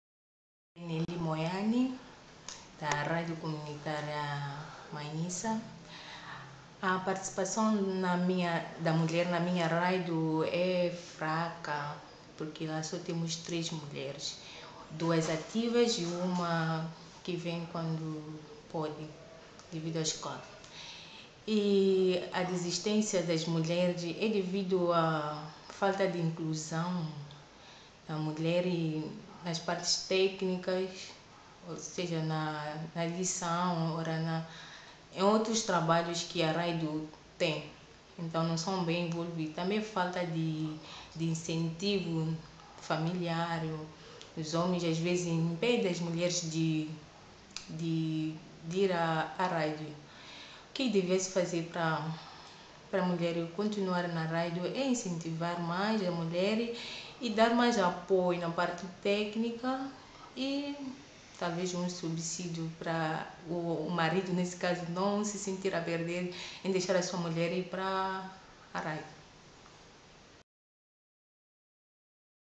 Participação da mulher nas rádios em Moçambique: prós e contras
Neli Moaina , do Centro Médio Comunitária de Manhiça falando do pós e contras da participação da mulher nas rádios em Moçambique